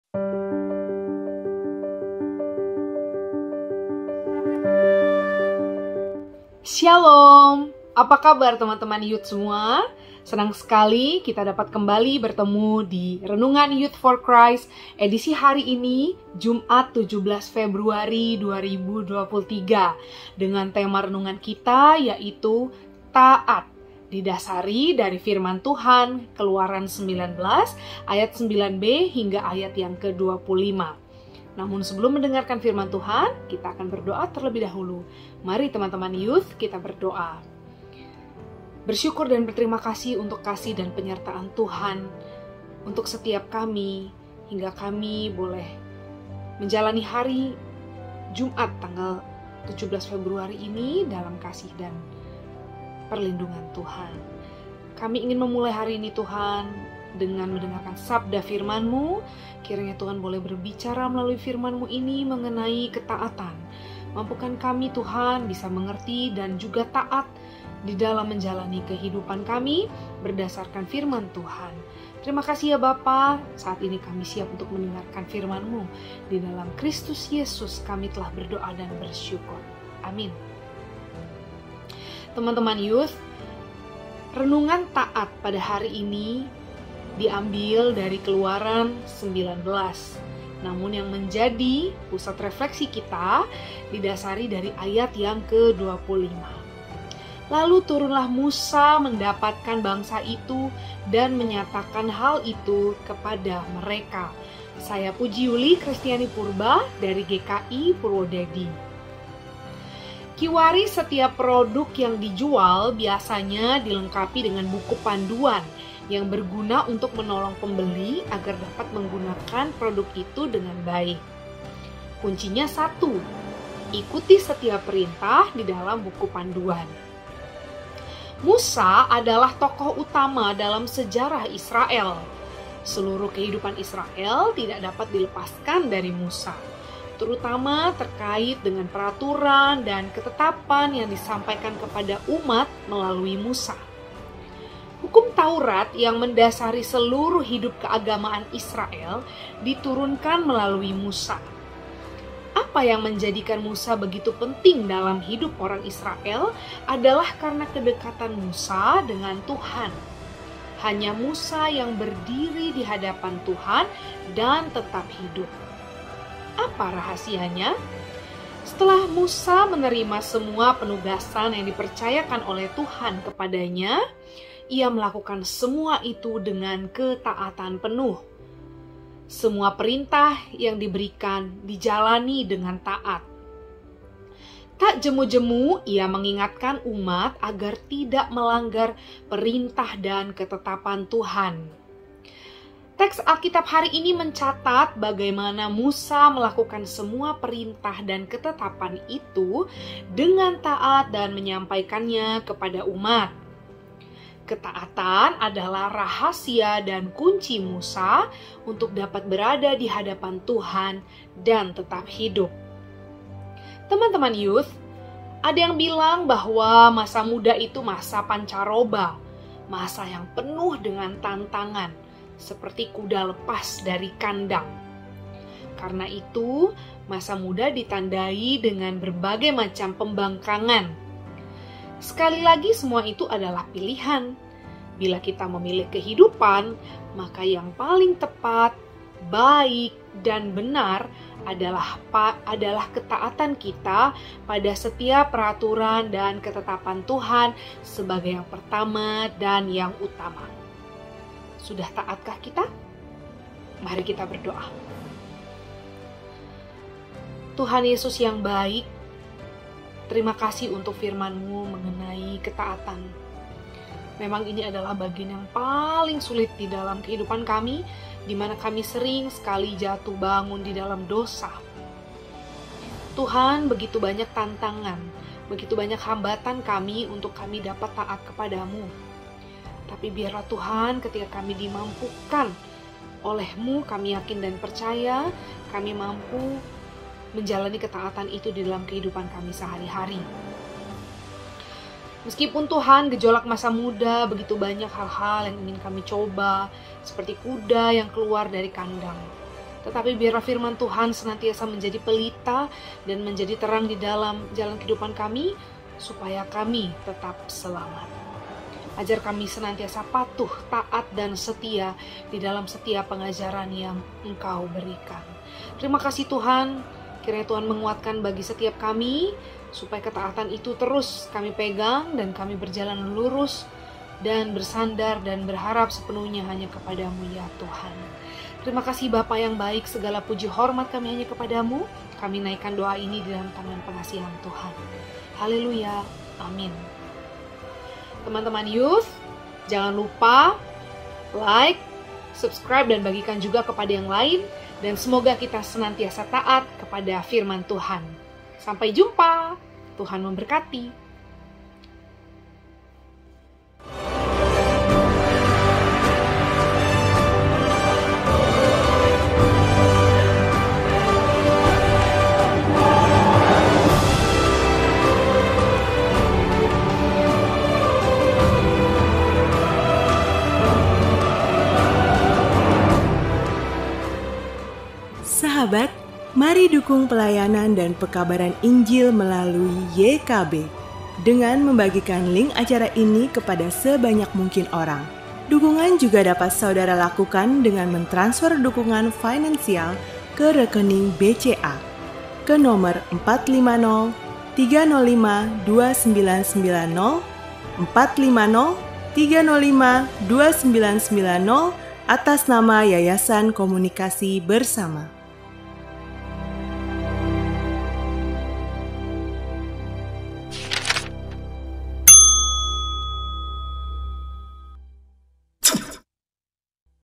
YOUTH FOR CHRIST 665 - TAAT
Renungan Harian Pemuda “Youth For Christ” untuk hari ini, Jumat, 17 Februari 2023
Berjudul : Taat
Didasarkan pada : Keluaran 19:9b-25
Dibawakan oleh : Puji Yuli Christiani Purba